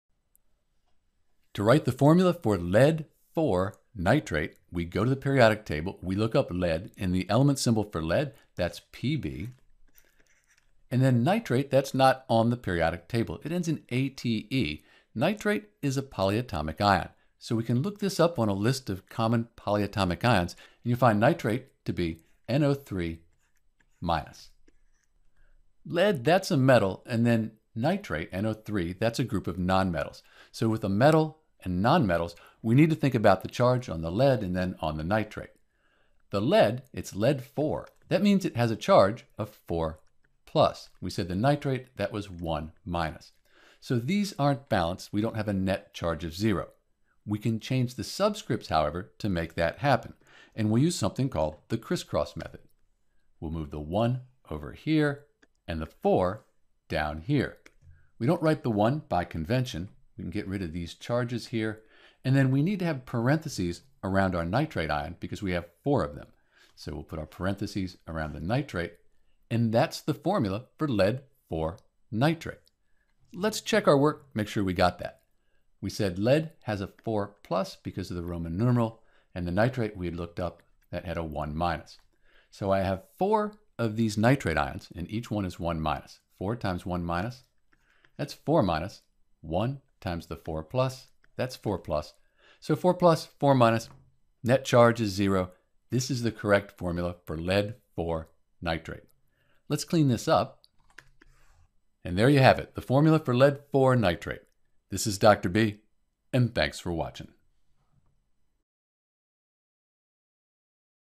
How to Write the Formula for Lead (IV) nitrate
In this video we'll write the correct formula for Pb(NO3)4, Lead (IV) nitrate.

To write the formula for Pb(NO3)4 we’ll use the Periodic Table, a Common Ion Table, and follow some simple rules.

Because Pb(NO3)4 has a polyatomic ion (the group of non-metals after the metal) we’ll need to use a table of names for common polyatomic ions, in addition to the Periodic Table.

1. Write the symbol and charge for the transition metal. The charge is the Roman Numeral in parentheses.
2. Write the symbol and charge for the non-metal. If you have a polyatomic ion, use the Common Ion table to find the formula and charge.
3. See if the charges are balanced (if they are you’re done!)
4. Add subscripts (if necessary) so the charge for the entire compound is zero.
5. Use the crisscross method to check your work.

For a complete tutorial on naming and formula writing for compounds, like Pb(NO3)4 and more, visit: